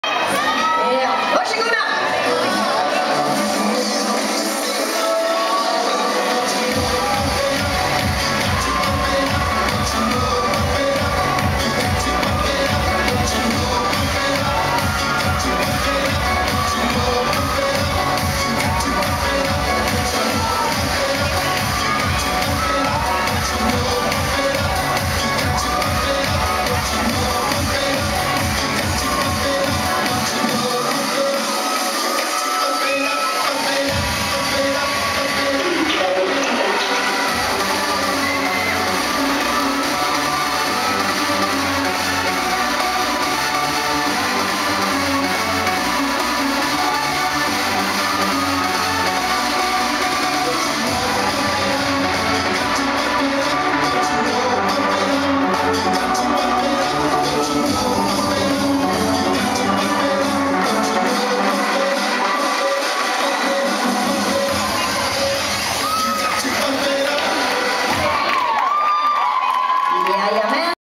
Amanda Wilson & Victoria Röhr/Jennie Hedman & Frida Lundberg - Kalmar 2012
Amanda Wilson & Victoria Röhr, Sinclairs, 4:a
Jennie Hedman & Frida Lundberg, ND/ÖDT, 5:a

Pardisco Superstar 16+